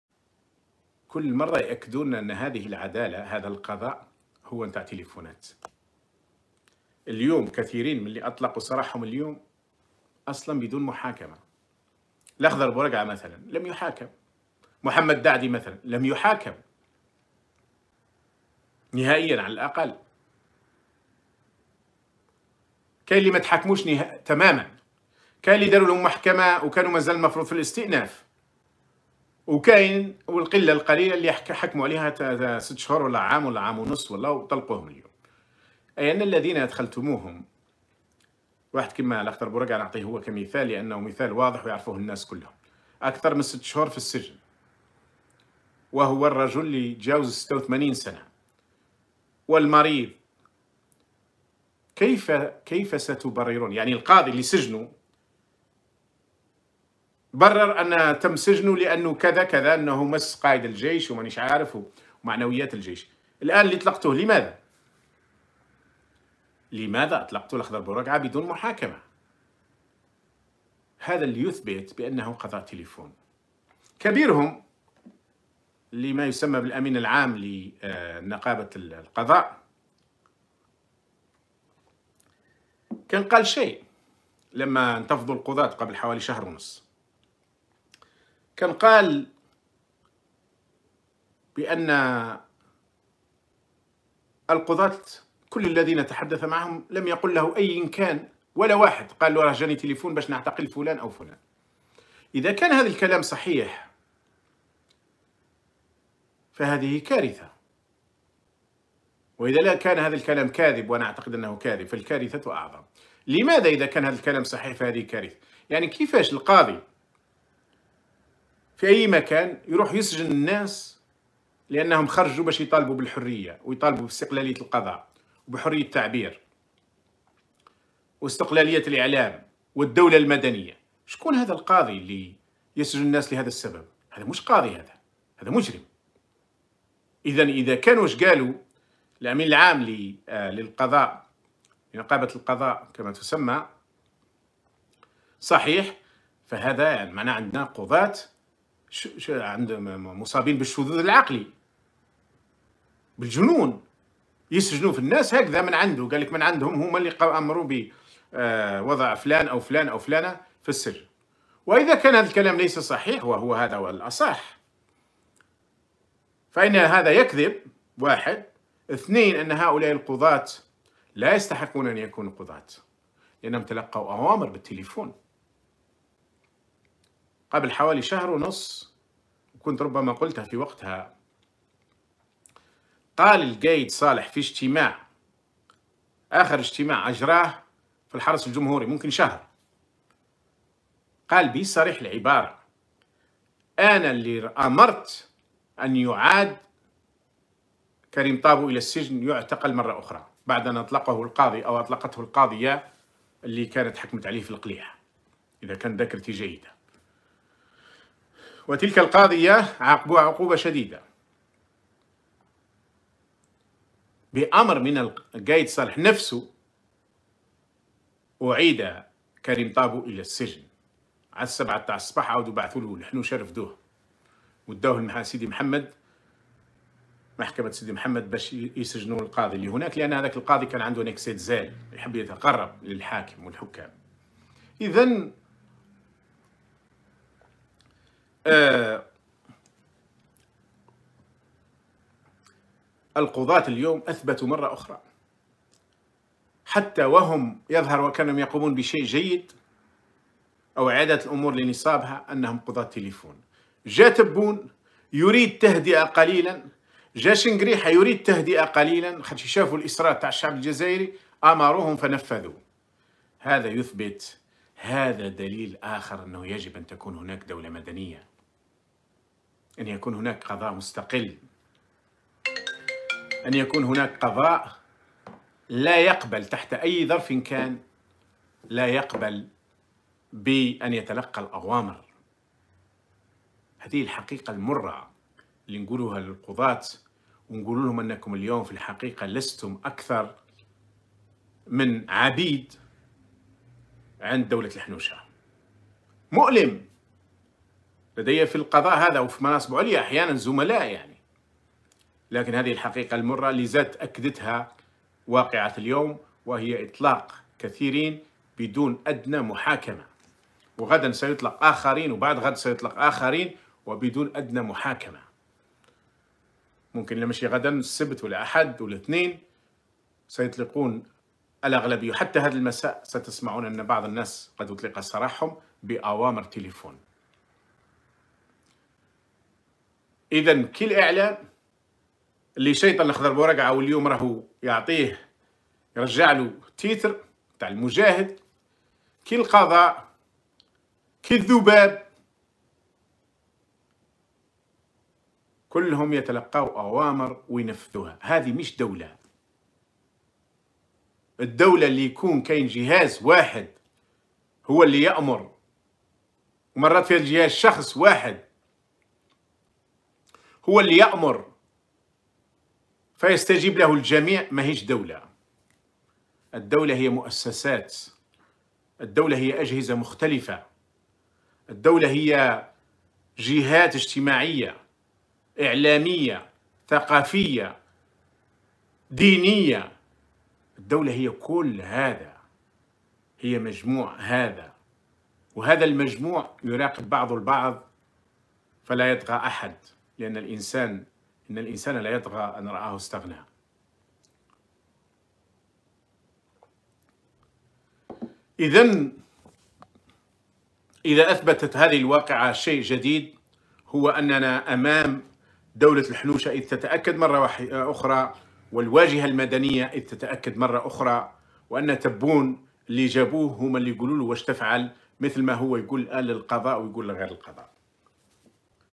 ' مرة أخرى يثبتون أن العدالة ' عدالة تلفون
الدولة التي يتحكم بها جهاز واحد ليست دولة
الدولة يجب ان تكون عبارة عن مؤسسات

زيتوت محمد العربي 02 يناير 2020